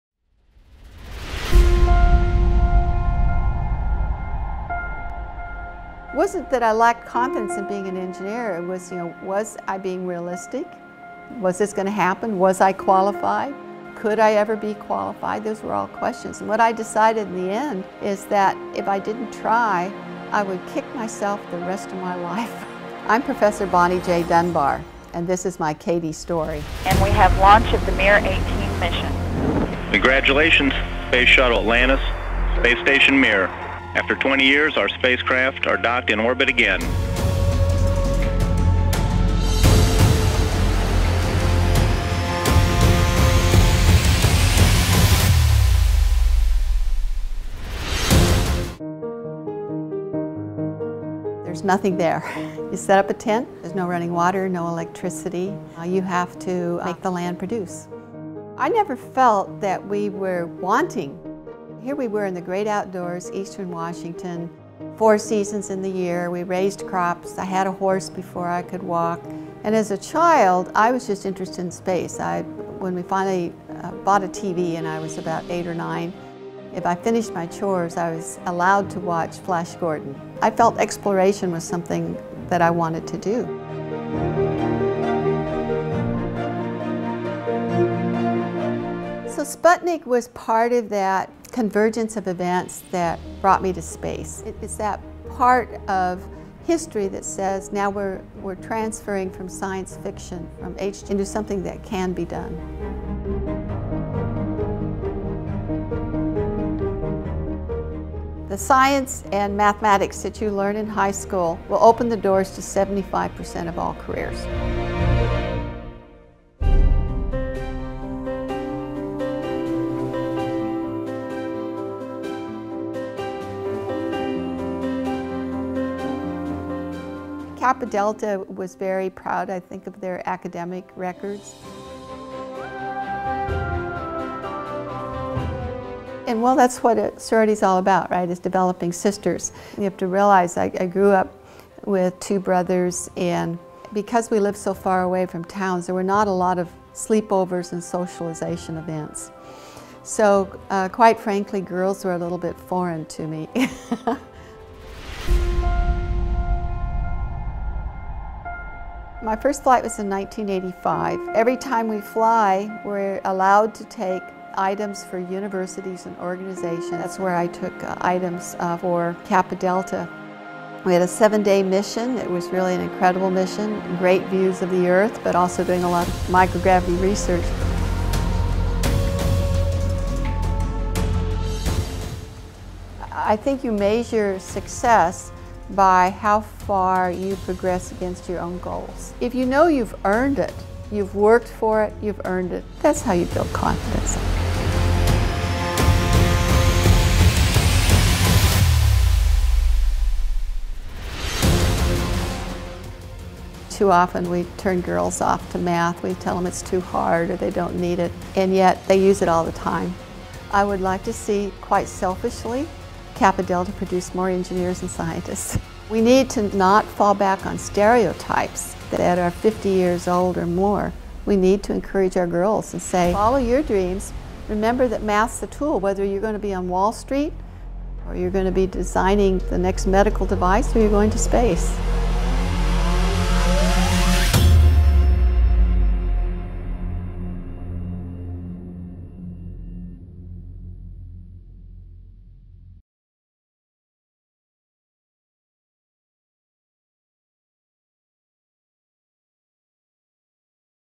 My KD Story: Bonnie Dunbar
Bonnie Dunbar shares her story of traveling the world and beyond - to space.